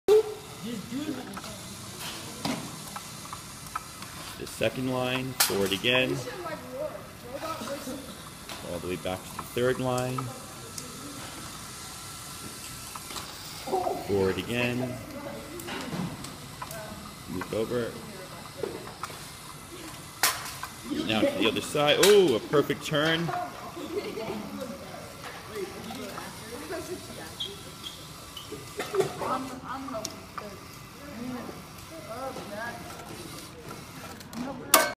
Emma Taylor
Students measured diameter, calculated circumference, and used circumference to determine the distance their robot would travel in one rotation. They then used this information to determine how far their robot would need to travel to knock things off the table, w/o falling off themselves, from specific distances.